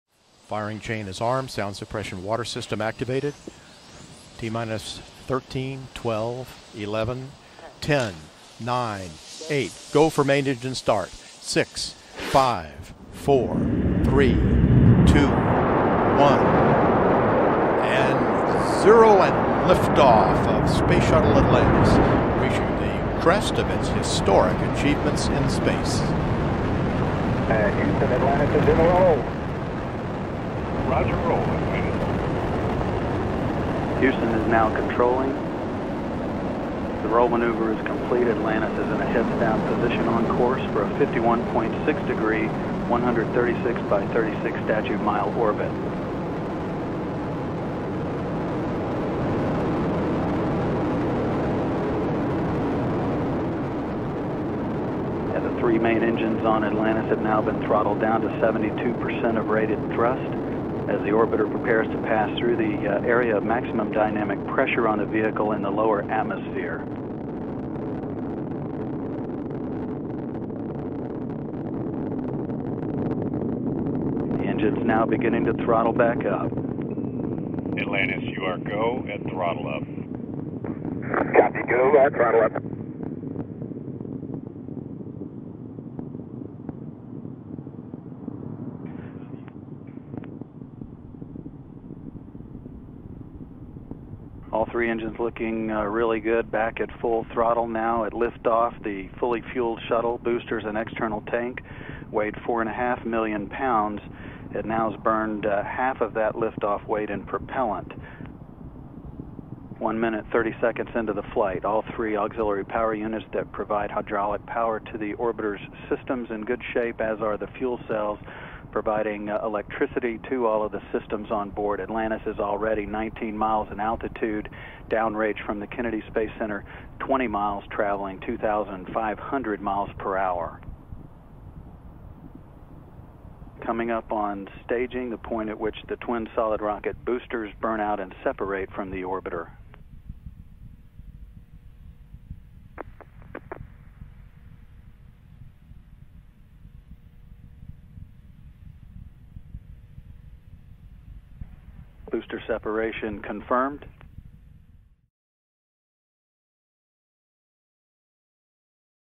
Atlantis Blasts Off! 🚀 | STS-132 Space Shuttle Liftoff 🌍
Space Shuttle Atlantis soars into the sky, marking the beginning of the STS-132 mission! 🚀 This historic launch carried a six-member crew toward the International Space Station (ISS), delivering the Russian Mini-Research Module-1 (MRM-1). It was one of Atlantis' final flights before retirement.

📅 Launch Date: May 14, 2010
🛰️ Mission: STS-132
👨‍🚀 Crew Members: 6
📍 Destination: International Space Station
🚀 Orbiter: Space Shuttle Atlantis
🎨 Image Credit: NASA
🎶 Audio: Original